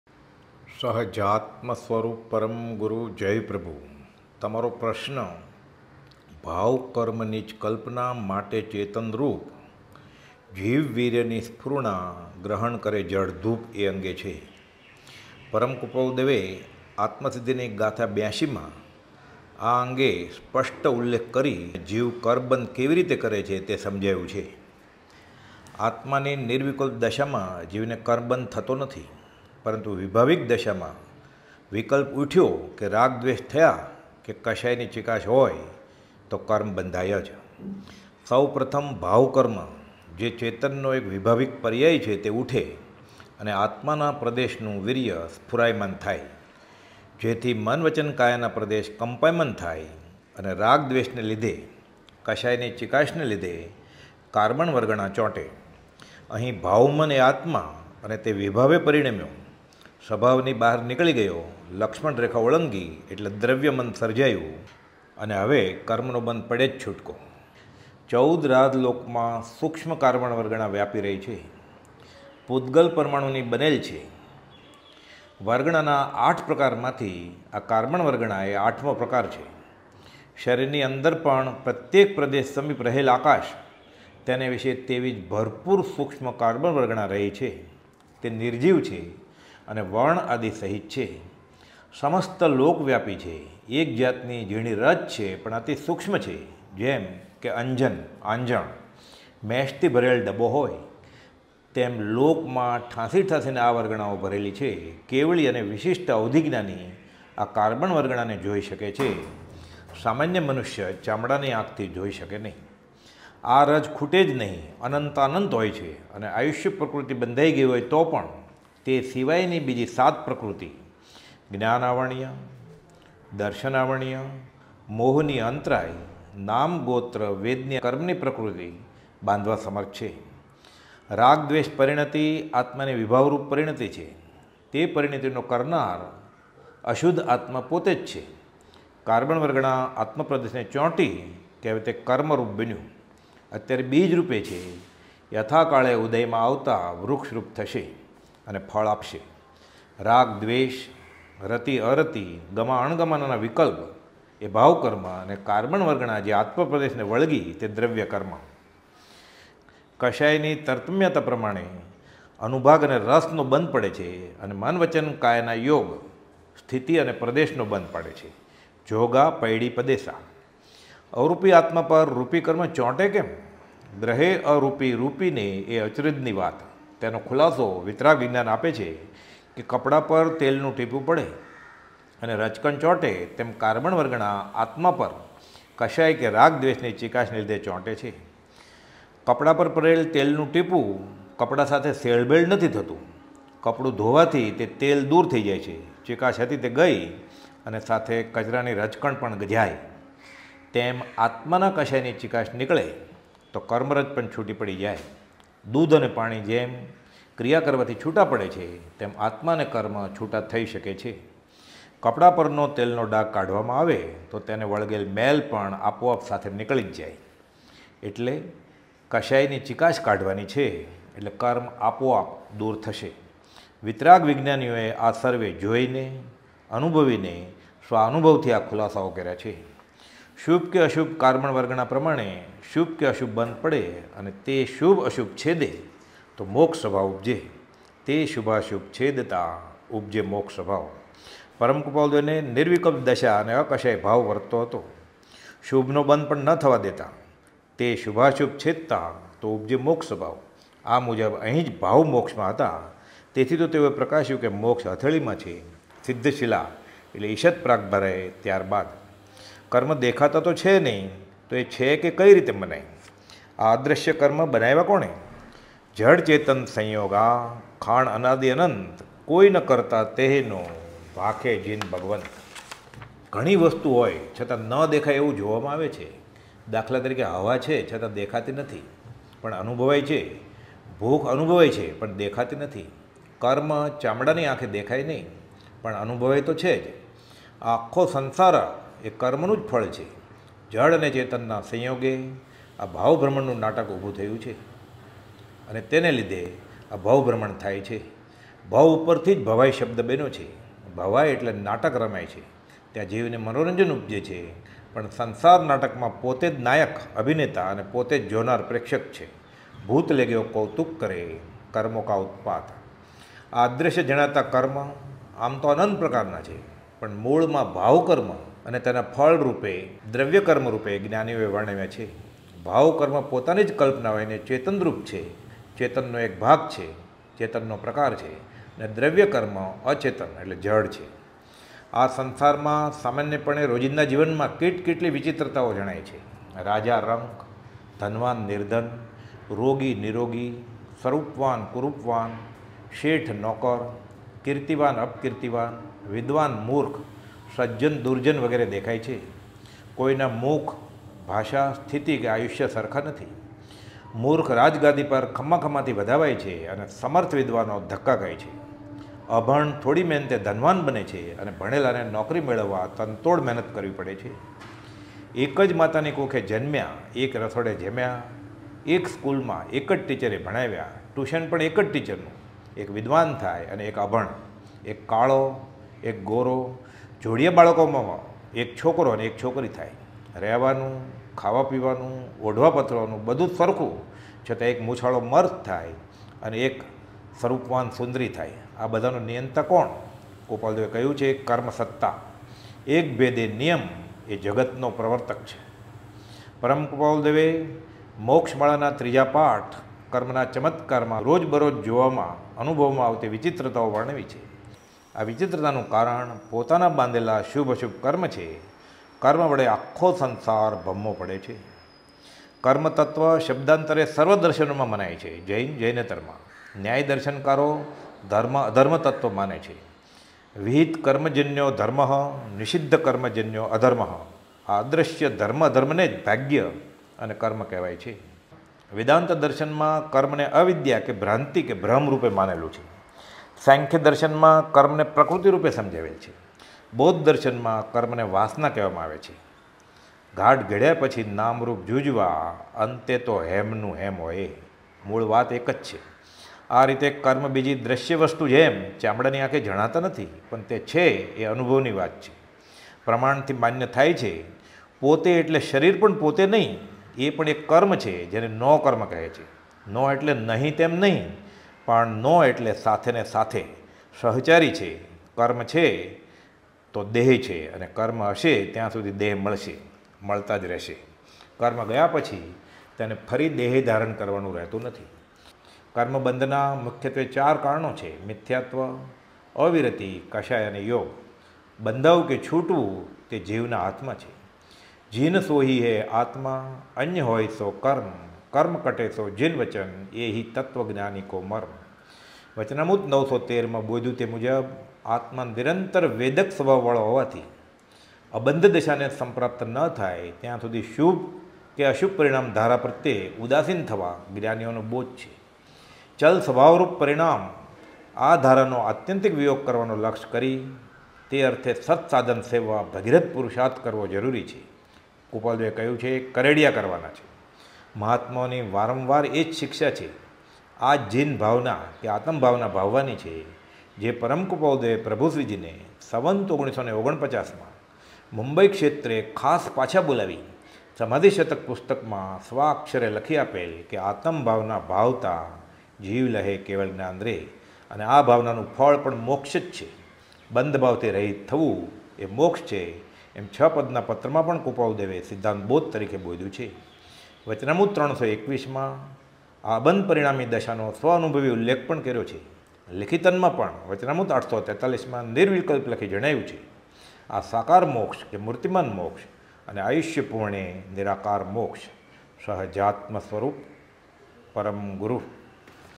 'ભાવકર્મ' - ચેતન રૂપ છે અને 'દ્રવ્યકર્મ' જડ છે- 'સંયોગ'નું ફળ સંસાર છે || Dr. Deepak Turakhia
શ્રી આત્મસિદ્ધિ શાસ્ત્ર - ગાથા - ૮૨